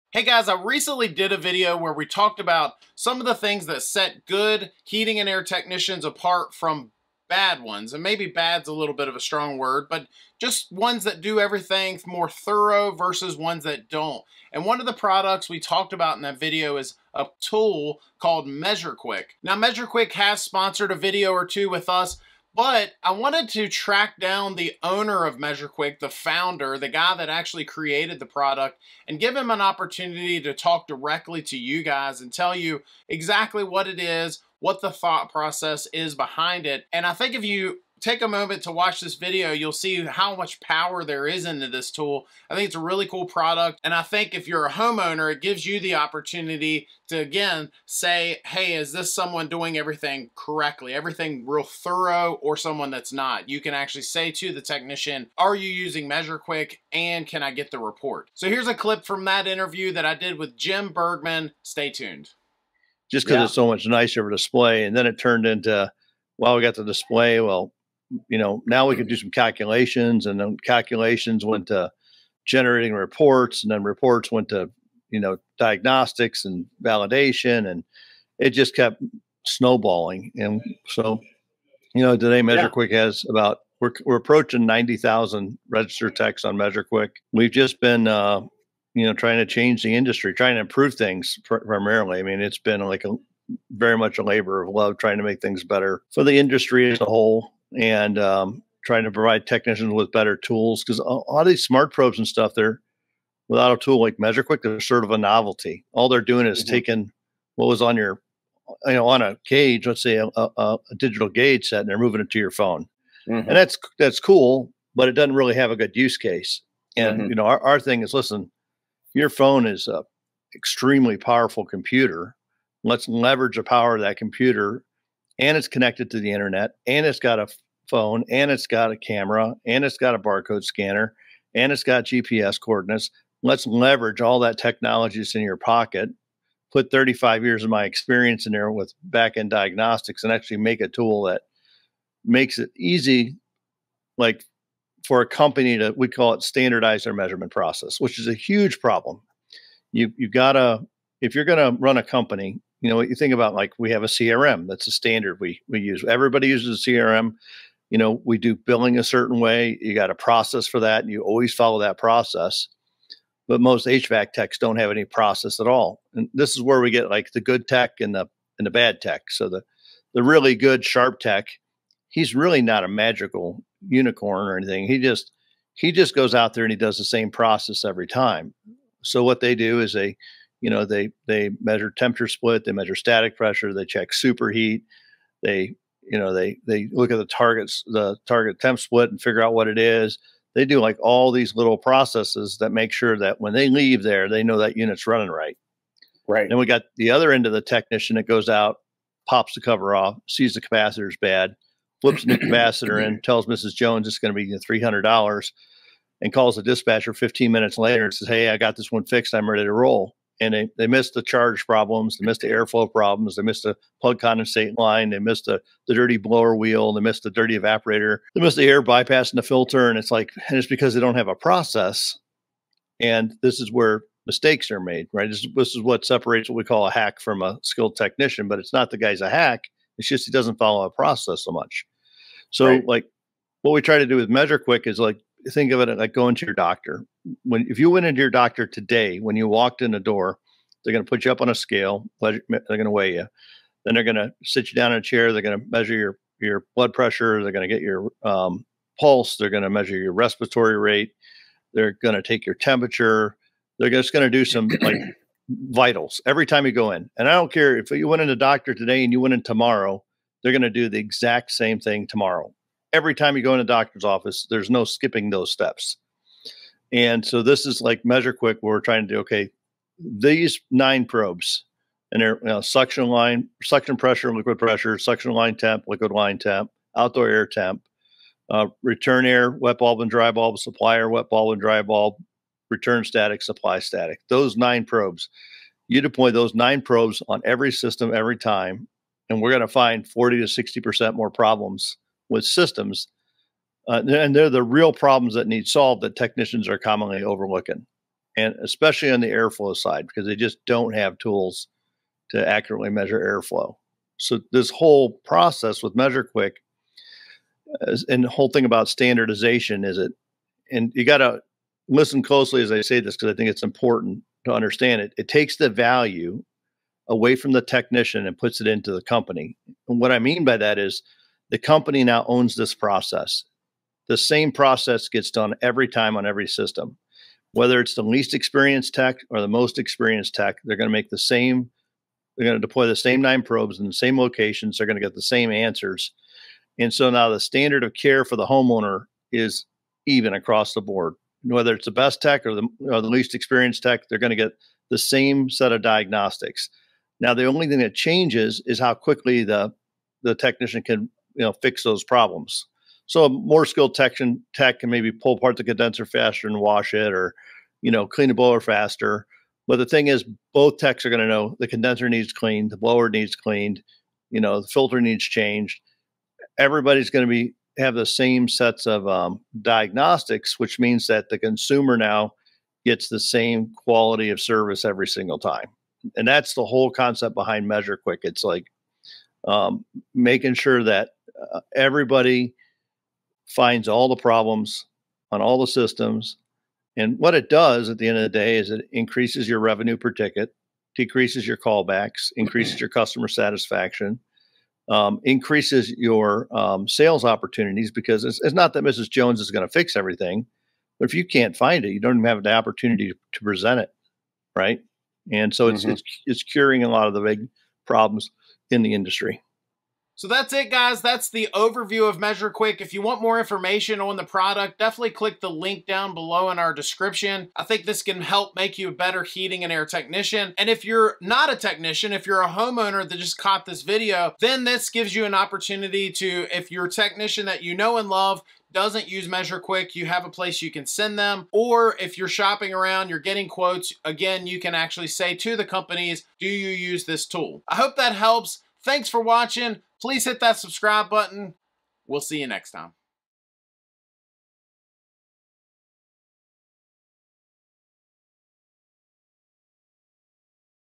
Making HVAC Techs Better! measureQuick Overview
In today's video, Joshua Griffin, interviews Jim Bergmann, the creator of measureQuick. Measure Quick, spelled measureQuick, is a tool that heating and air professionals can use to make sure they are getting the job done right and diagnosing systems properly. Whether the system is new during start-up or a system that has something going wrong, MeasureQuick can help an HVAC technician find the problem and make the necessary repairs, saving the homeowner money, in the long run, knowing that all bases have been covered. Jim gives an overview of Measure Quick and the benefits of why every HVAC professional should be using this product!

Chapters
0:00 Intro: MeasureQuick
0:22 Owner of MeasureQuick Intro
1:03 Jim Bergmann interview
3:54 MeasureQuick Thought Process
6:50 Outro and Next Steps